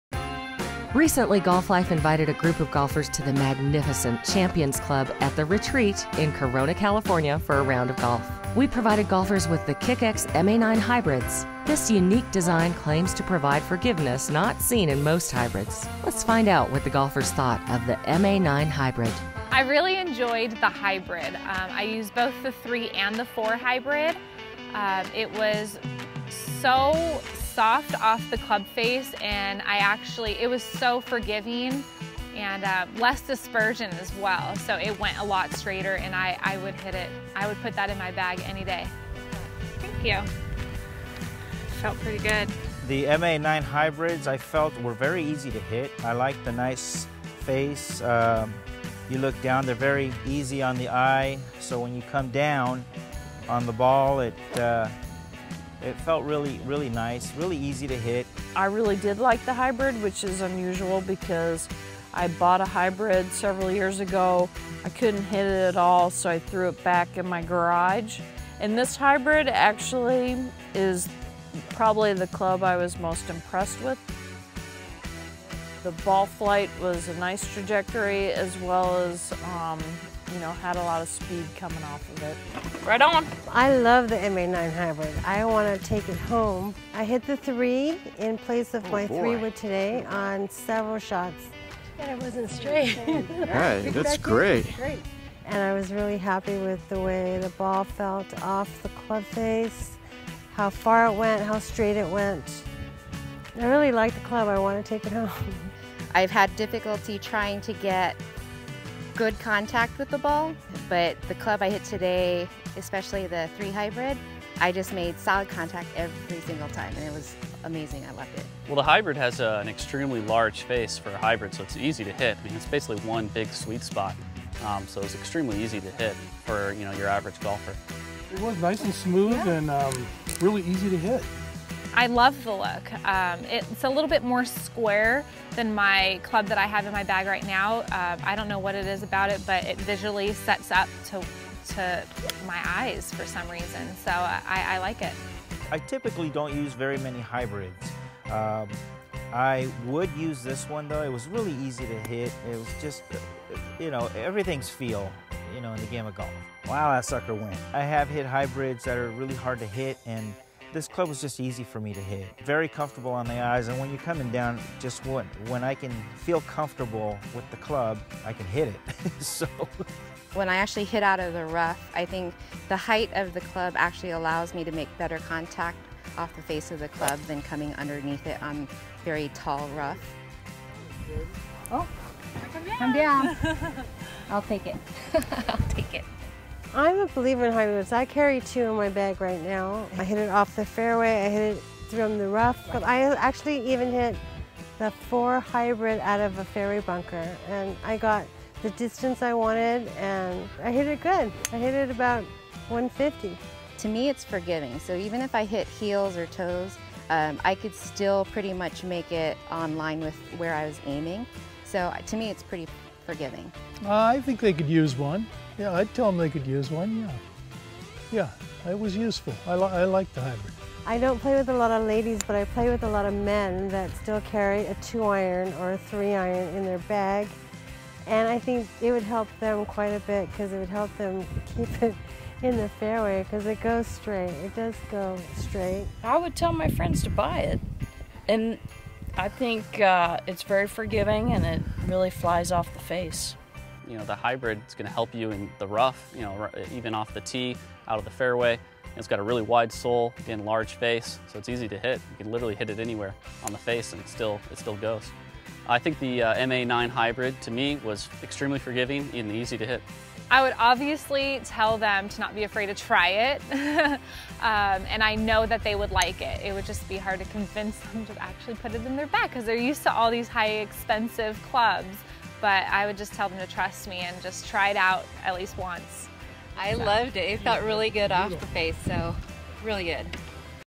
MA-Nine Hybrids
Golf Life invited a group of golfers to the magnificent Champions Club at The Retreat in Corona, California for a round of golf. The Champions Club is a beautiful golf resort and provides a perfect backdrop to test new golf clubs. We provided golfers with the Kick X MA-nine Hybrids. This unique design claims to provide forgiveness not seen in most hybrids.

The MA-NINE hybrids are the easiest clubs to hit in any condition from tight lies to thick rough. Unique crown design promotes precision alignment and helps with address. Built with a hotter face and one of the biggest sweetspot hybrids can offer, the MA-NINE 's are designed to provide the confidence, distance, & accuracy every golfer needs.

Experience amazing accuracy, unmatched distance and maximum sweet-spot forgiveness.

TRY THE
TODAY, RISK FREE!